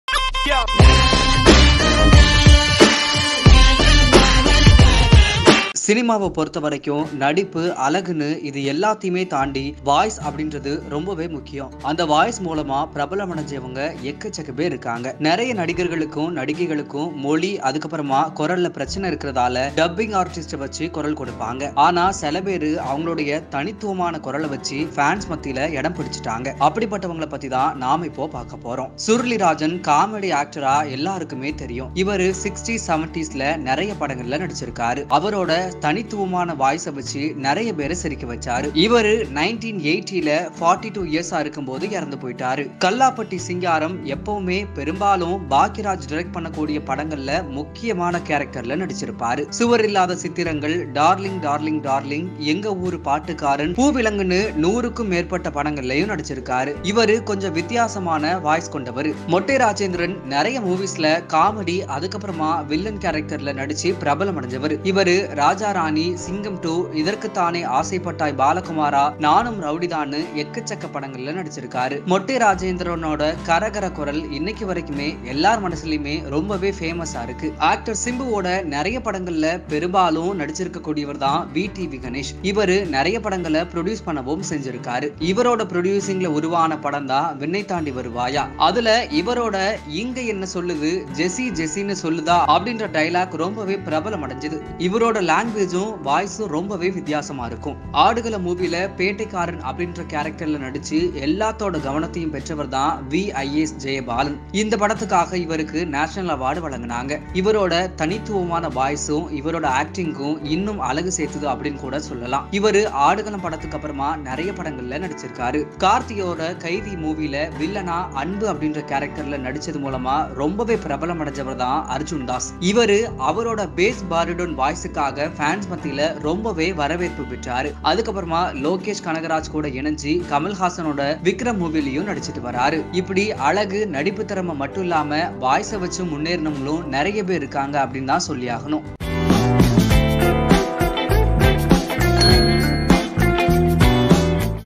விசித்திரமான குரலை வைத்து வெற்றிகண்ட 6 நடிகர்கள்.. மிரட்டியே வாய்ப்பை அள்ளிய அர்ஜுன் தாஸ்
சினிமாவைப் பொருத்தவரை நடிப்பு, அழகு ஆகியவற்றை தாண்டி குரல் வளத்தினால் பிரபலமடைந்தவர்கள் உள்ளனர். சில நடிகர், நடிகைகளுக்கு மொழி மற்றும் குரல் பிரச்சனைகளால் டப்பிங் ஆர்டிஸ்ட் வைத்த குரல் கொடுக்கப்படுகிறது. ஆனால் சிலர் தங்களுடைய தனித்துவமான குரலால் ரசிகர்களின் மனதில் இடம் பிடித்துள்ளனர்.

சுருளிராஜன் : சுருளிராஜன் நகைச்சுவை நடிகராக அனைவராலும் அறியப்படுபவர். இவர் 60, 70 களில் பல படங்களில் நடித்திருந்தார். தன்னுடைய தனித்துவமான குரலால் பலரையும் சிரிக்க வைத்தார். புகழின் உச்சத்தில் இருக்கும் போதே தனது 42வது வயதில் 1980 இல் காலமானார்.

கல்லாப்பெட்டி சிங்காரம் : இவர் பெரும்பாலும் பாக்யராஜ் இயக்கும் திரைப்படங்களில் முக்கியமான கதாபாத்திரத்தில் நடித்திருப்பார். சுவரில்லாத சித்திரங்கள், டார்லிங் டார்லிங் டார்லிங், எங்க ஊரு பாட்டுக்காரன், பூவிலங்கு போன்ற நூற்றுக்கும் மேற்பட்ட திரைப்படங்களில் நடித்துள்ளார். இவர் சற்று வித்தியாசமான குரல் வளம் உடையவர்.
மொட்டை ராஜேந்திரன் : இவர் பெரும்பாலான படங்களில் நகைச்சுவை மற்றும் வில்லன் கதாபாத்திரங்களில் நடித்து பிரபலமானவர். இவர் ராஜா ராணி, சிங்கம் 2,இதற்குத்தானே ஆசைப்பட்டாய் பாலகுமாரா, நானும் ரவுடிதான் போன்ற படங்களில் நடித்துள்ளார். மொட்டை ராஜேந்திரன் கரகர என்ற குரல் வளம் உடையவர்.

விடிவி கணேஷ் : நடிகர் சிம்புவின் பெரும்பாலான படங்களில் நடித்திருப்பவர் விடிவி கணேஷ். இவர் சில படங்களை தயாரித்தும் உள்ளார். இவருடைய தயாரிப்பில் உருவான விண்ணைத்தாண்டி வருவாயா படத்தில் அனைவருக்கும் பரிச்சயமானர். அதில் இவருடைய இங்க என்ன சொல்லுது ஜெசி ஜெசி சொல்லுடா என்ற வசனம் மிகவும் பிரபலம். இவருடைய பாஷை மற்றும் குரல் வித்தியாசமாக இருக்கும்.

விஐஎஸ் ஜெயபாலன் : ஆடுகளம் படத்தில் பேட்டைக்காரன் என்ற கதாபாத்திரம் உள்ள அனைவரது கவனத்தையும் ஈர்த்தவர் ஜெயபாலன். இப்படத்திற்காக இவருக்கு தேசிய விருதும் கிடைத்தது. இவருடைய தனித்துவமான குரலும் இவர் நடிப்புக்கு இன்னும் அழகு சேர்த்தது. இவர் ஆடுகளம் படத்தை தொடர்ந்து பல படங்களில் நடித்துள்ளார்.
அர்ஜுன் தாஸ் : கார்த்தியின் கைதி படத்தில் வில்லனாக அன்பு கதாபாத்திரத்தில் நடித்ததன் மூலம் மிகவும் பிரபலமானவர் அர்ஜுன் தாஸ். இவர் தனது பேஸ் பாரிடோன் குரலுக்காக ரசிகர்களின் கவனத்தைப் பெற்றார். பிறகு மீண்டும் லோகேஷ் கனகராஜ் உடன் இணைந்த கமலஹாசனின் விக்ரம் படத்தில் நடித்துள்ளார்.

starts: 0:00
second: 0:06
mid: 1:15
end: 2:20